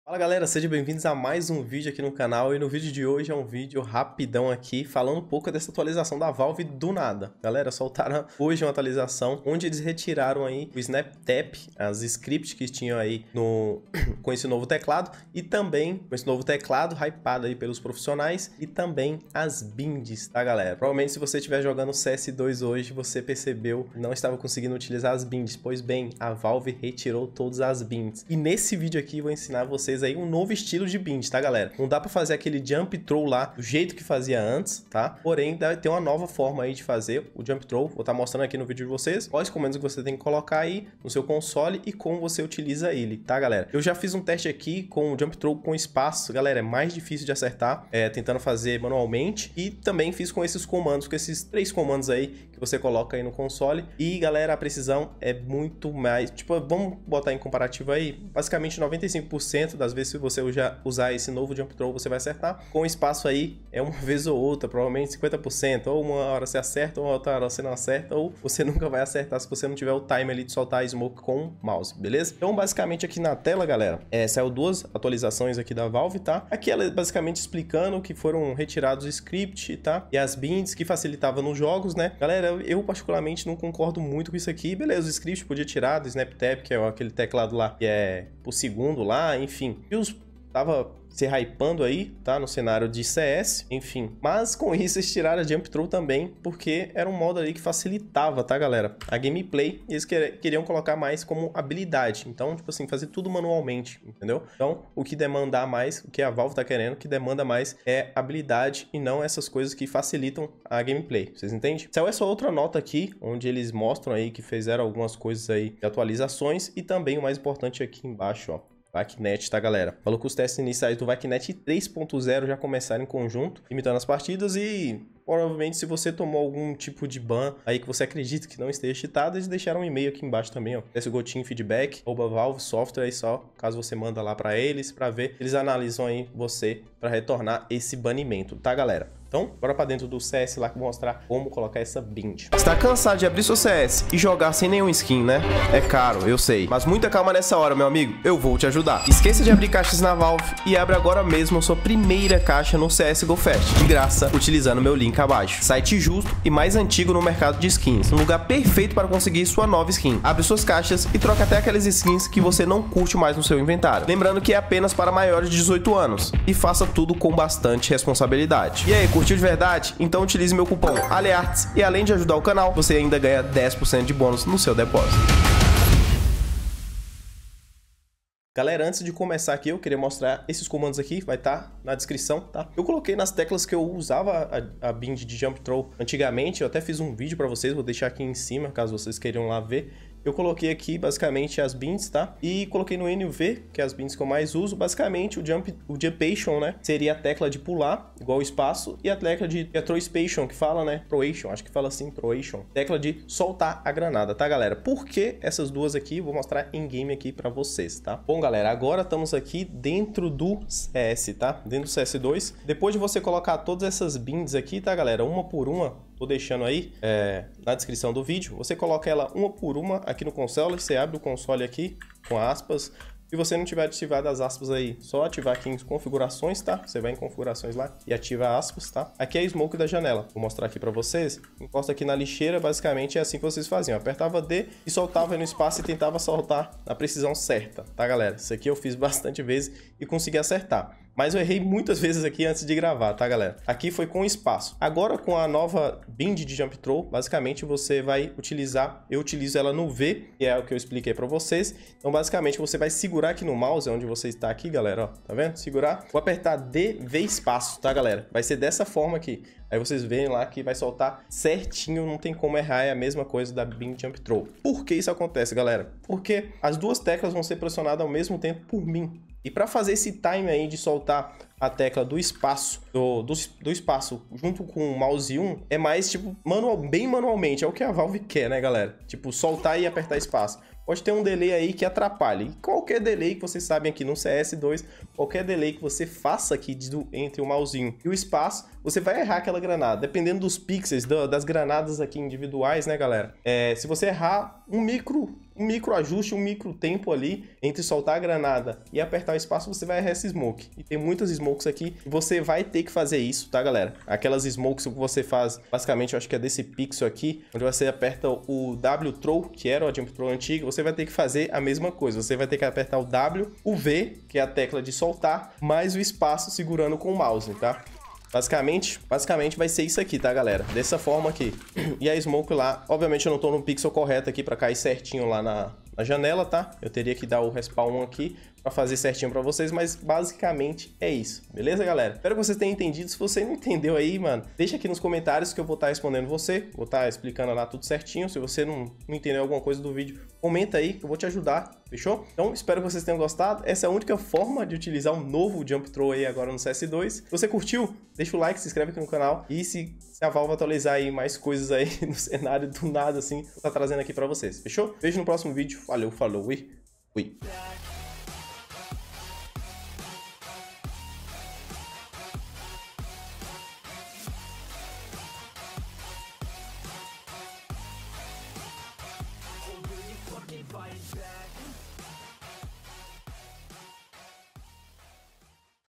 VALVE ATUALIZOU!! APRENDA a NOVA BIND de JUMP THROW 2024! (vac 3.0 funcionando...)
📚 Capitulos:
0:00 - Atualização da valve vac 3.0
03:28 - Explicando como funciona o Jumpthrow
05:32 - Finalização

🎮 Binds abaixo:
alias "+jumpaction" "+jump;"
alias "+throwaction" "-attack"
alias "-jumpaction" "''-jump"
bind n "+jumpaction"
bind v "+throwaction"
bind k "-jumpaction"

Ps:
jumpaction - tecla que pula igual espaço
throwaction - tecla para soltar a granada

Curtiu o conteúdo? então continue assistindo! ;)